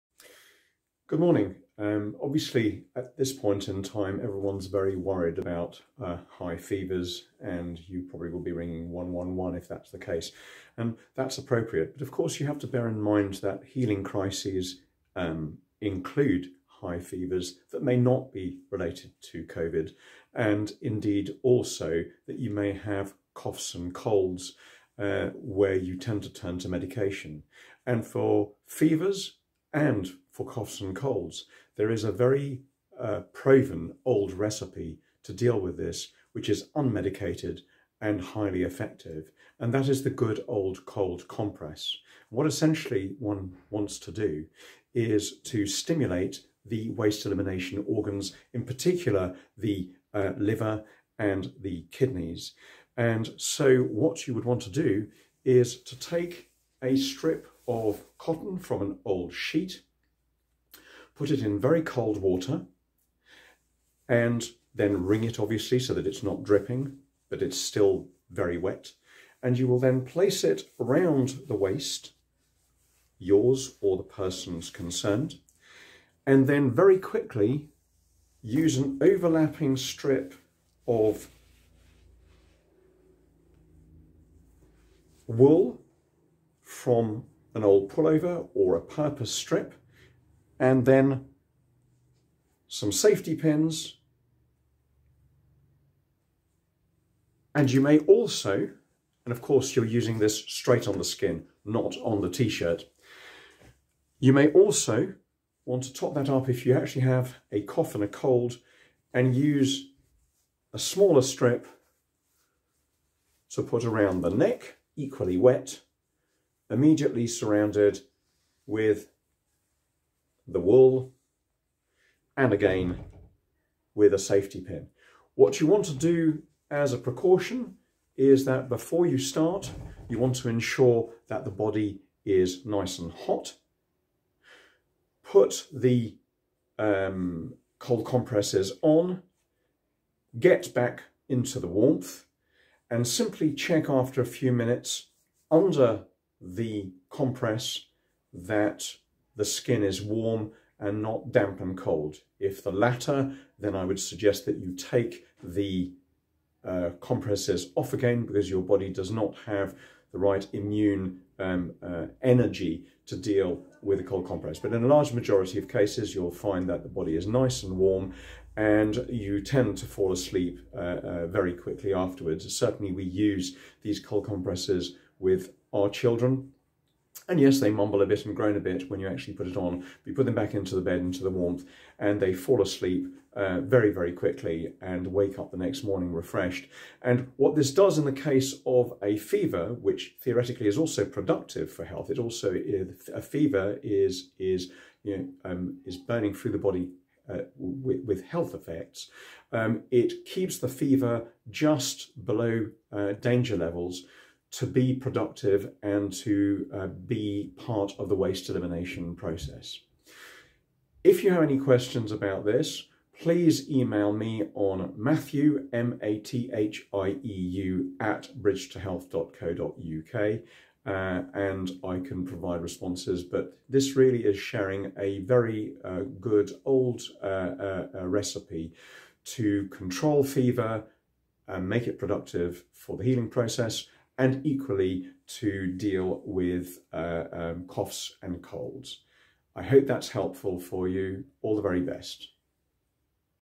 Osteopath Mathieu Rossano explains the benefits of cold compresses and explains how to make one
In the current context of self-isolation, a sudden high fever, or cough and cold might cause you to ring 111, and that is certainly appropriate. However, fevers and coughs will occur for many other attributable reasons than Covid19, and one time-honoured, effective and harmless form of treatment is the humble cold compress. Osteopath Mathieu Rossanon shares in this video how to use cold compresses to trigger effective waste elimination and symptomatic relief.